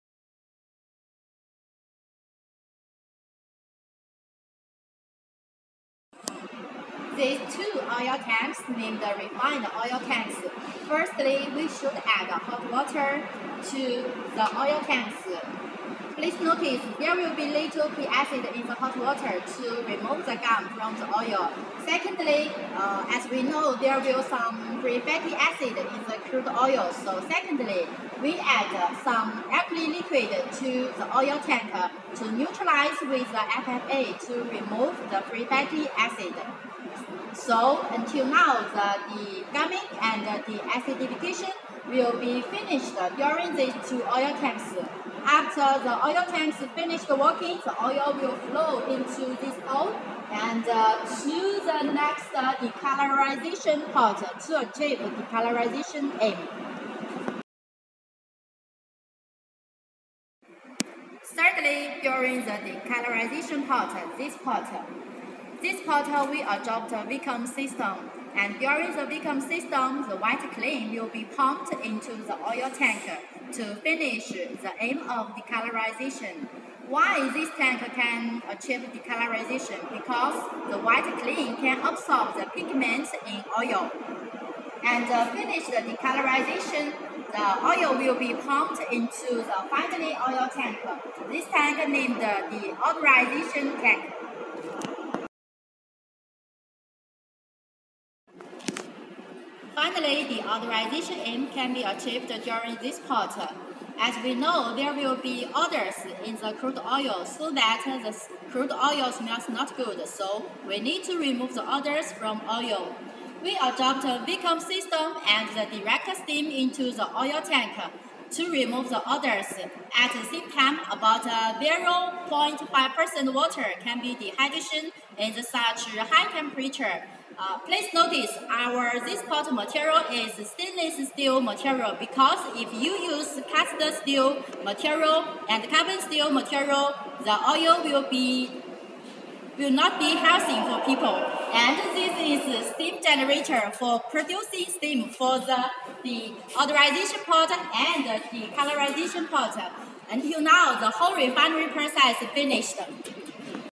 Small scale/mini sunflower oil refining machine, batch type sunflower oil refienry plant
Monica, our sales introduce the small scale/mini sunflower oil refining machine, batch type sunflower oil refienry plant in detail. In this video, you can hear the sunflower oil refining process in batch type sunflower oil refinery plant. Henan Doing Company can also provide large capacity sunflower oil refinery turn-key serives.